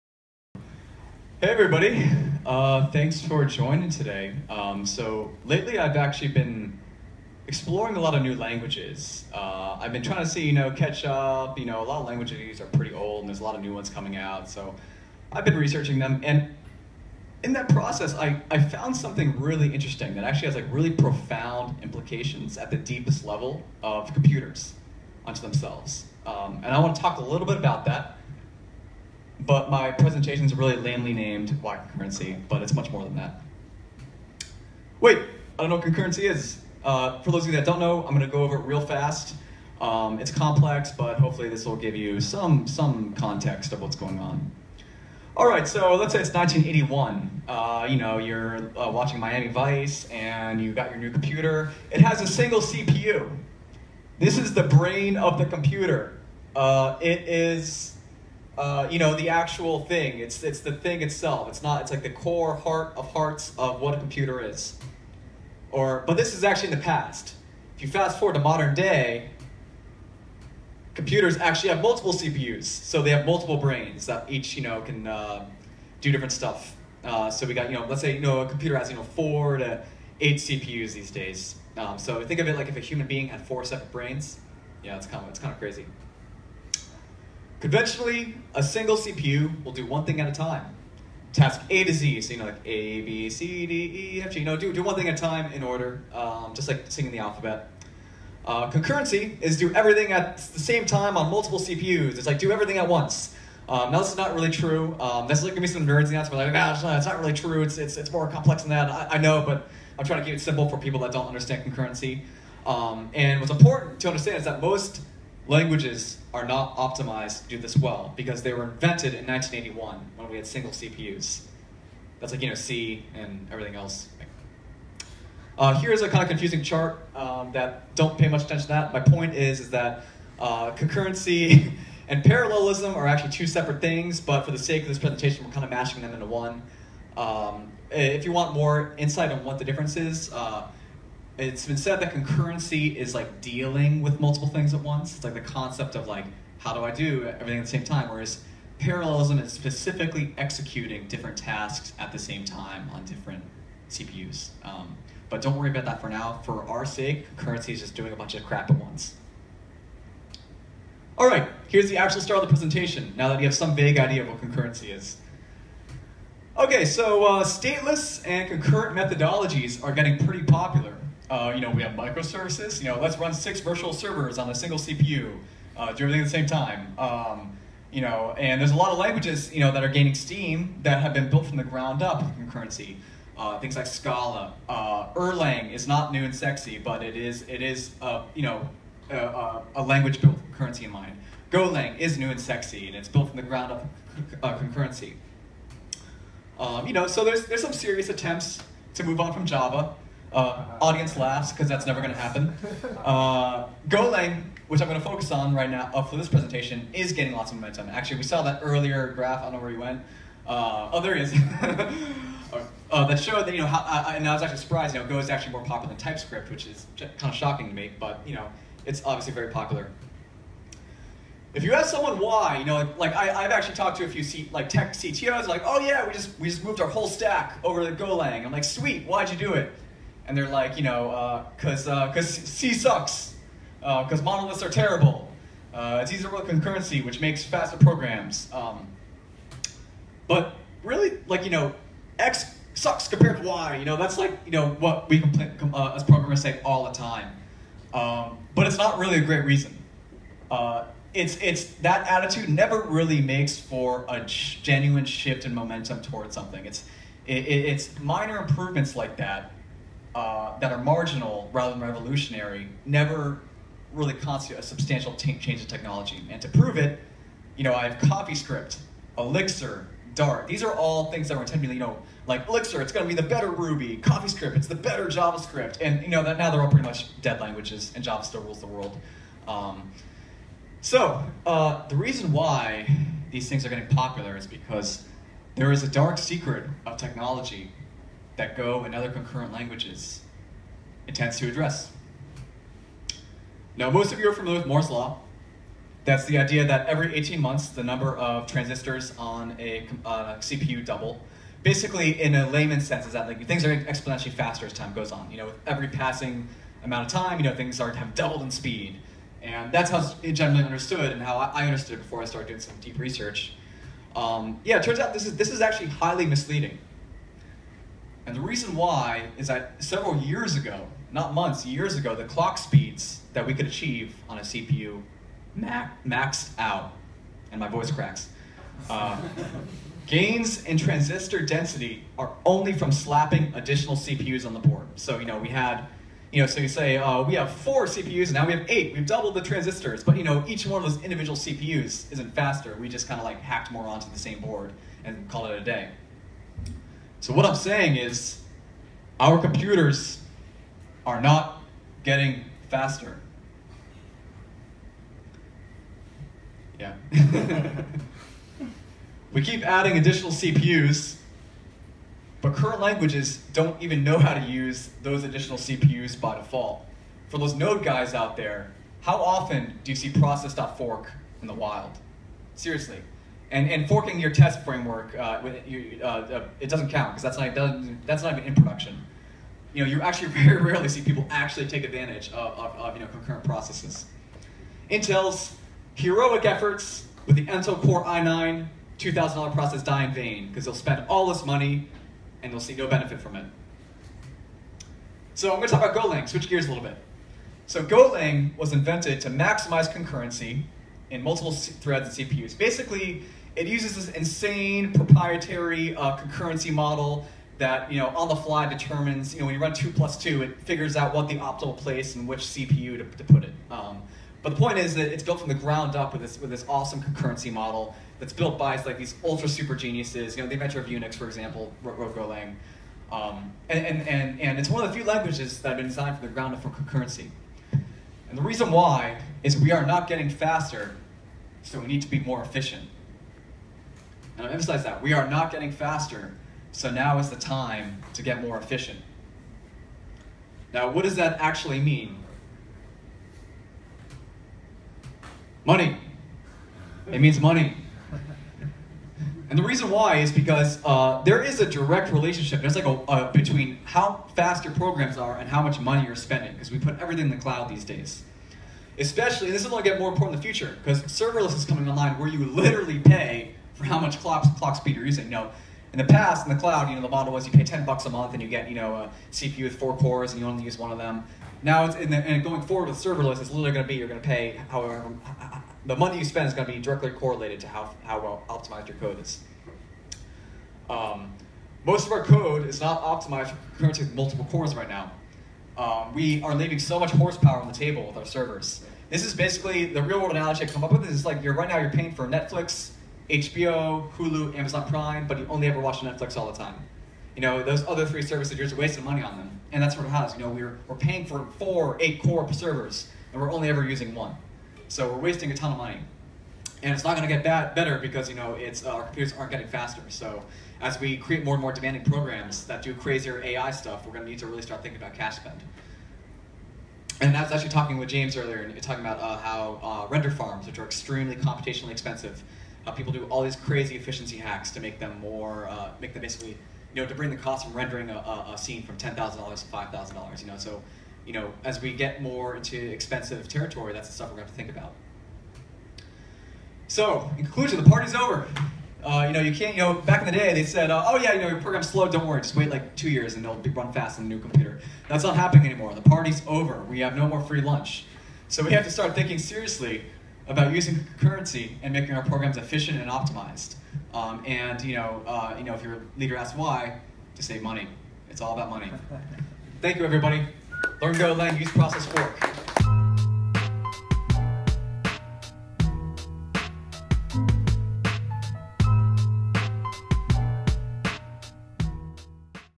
Why Concurrency? The Dark Secret of Tech | Will Bratches
Learn about Golang, the future, concurrency, and macro trends shaping the industry now with InRhythm's Will Bratches.

Join us on Meetup and be the first to know about our next InRhythmU event